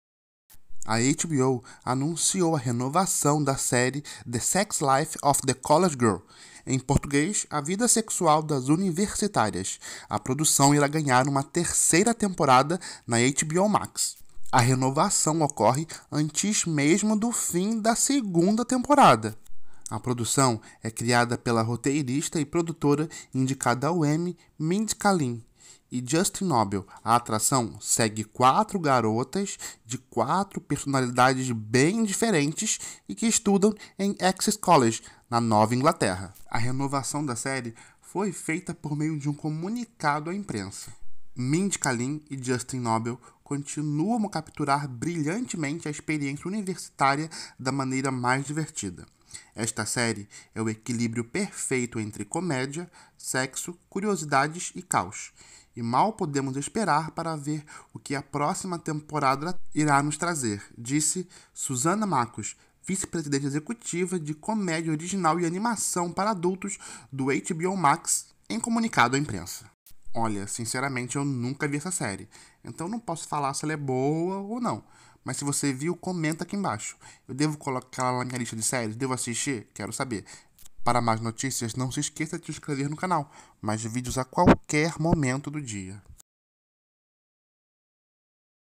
SÉRIE 'A VIDA SEXUAL DAS UNIVERSITÁRIAS' É RENOVADA PARA TERCEIRA TEMPORADA PELA HBO
A HBO renovou a série A Vida Sexual das Universitárias para a terceira temporada. A segunda temporada da série de comédia estreou em novembro deste ano.